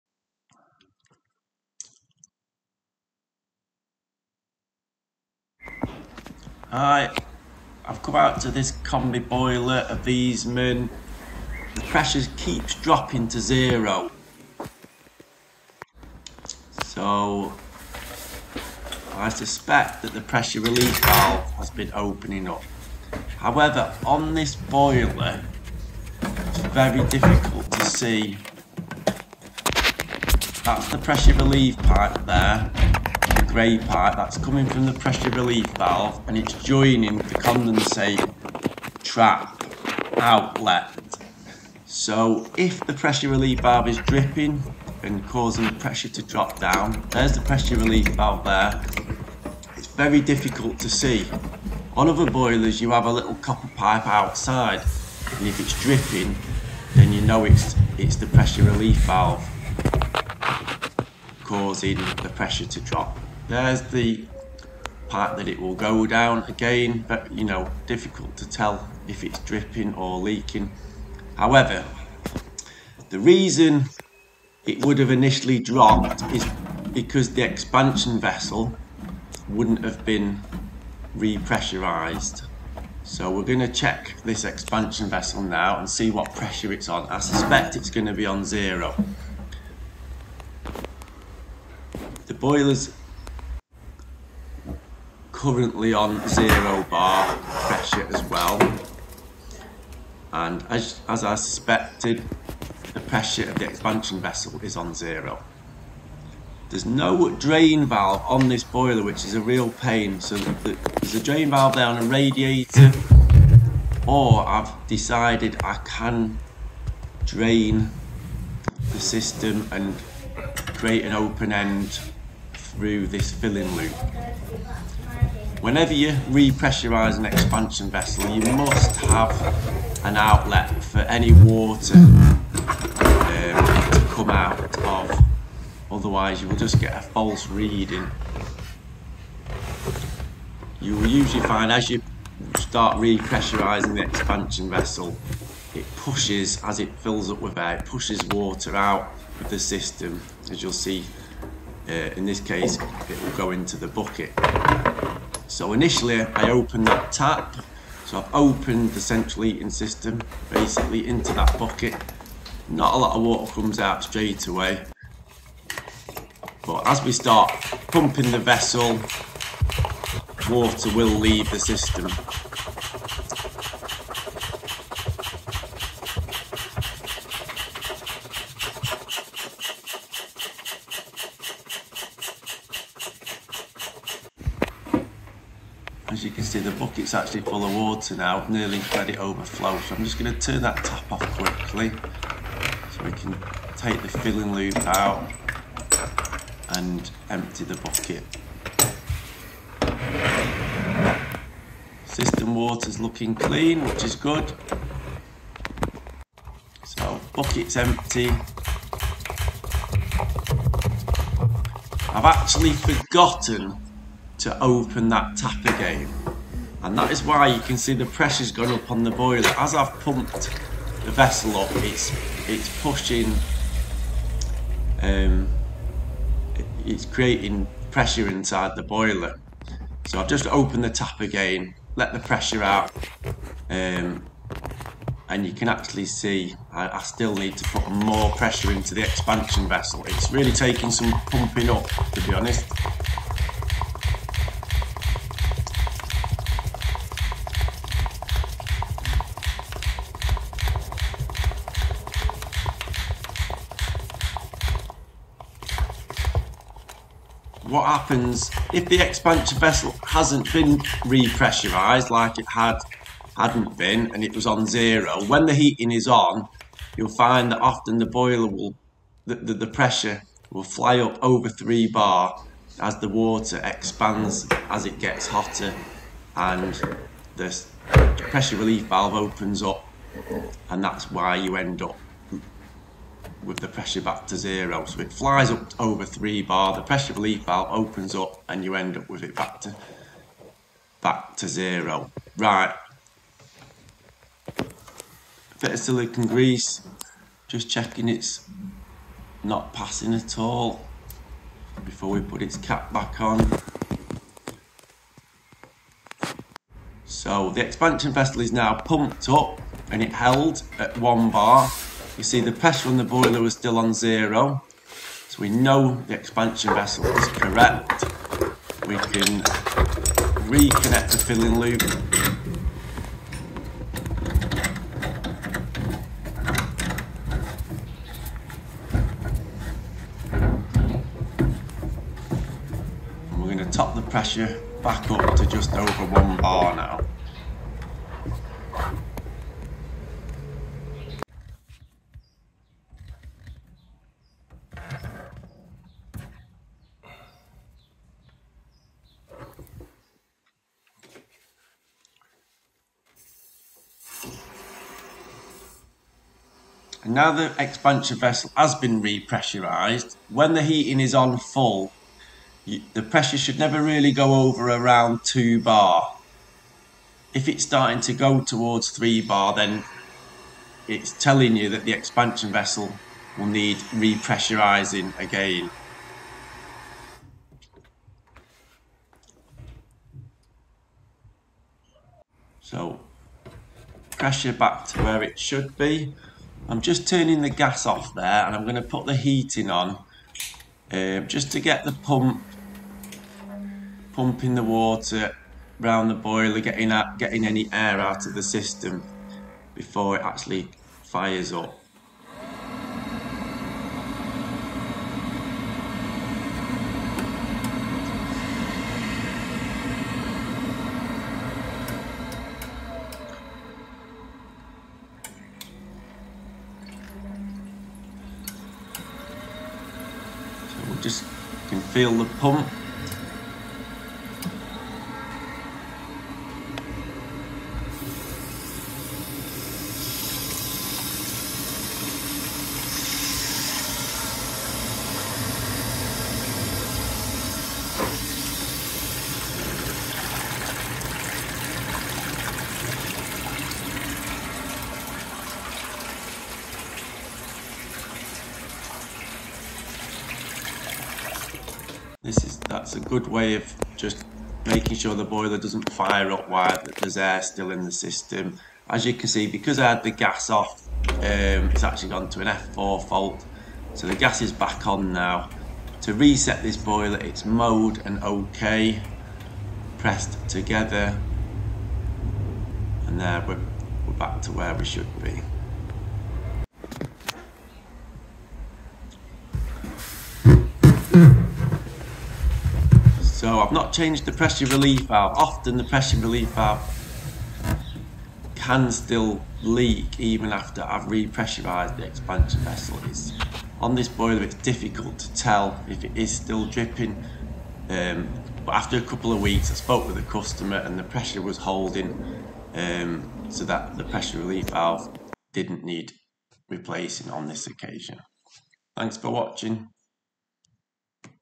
Viessmann boiler. Pressure on zero. Re pressurising the expansion vessel. No drain tap.
Found this Viessmann combi boiler quite un user friendly when having to re pressurise the expansion vessel